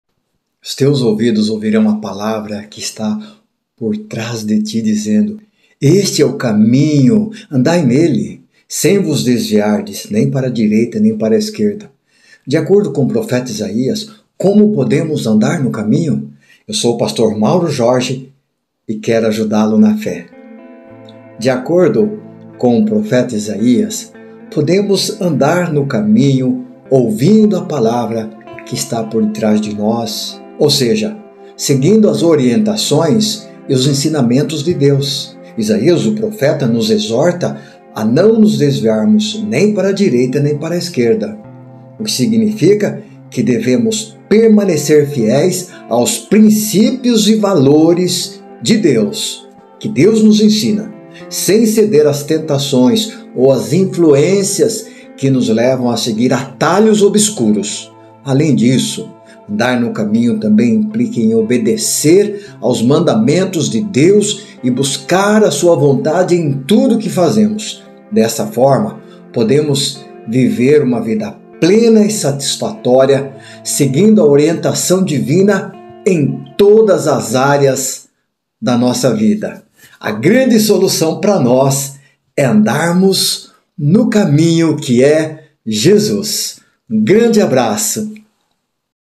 Qual é o caminho?
De acordo com o profeta Isaías, podemos andar no caminho ouvindo a palavra que está por trás de nós, ou seja, seguindo as orientações e os ensinamentos de Deus. Isaías nos exorta a não nos desviarmos nem para a direita nem para a esquerda, o que significa que devemos permanecer fiéis aos princípios e valores que Deus nos ensina, sem ceder às tentações ou influências que nos levam a seguir um caminho diferente. Além disso, andar no caminho também implica em obedecer aos mandamentos de Deus e buscar a Sua vontade em tudo o que fazemos. Dessa forma, podemos viver uma vida plena e satisfatória, seguindo a orientação divina em todas as áreas da nossa vida.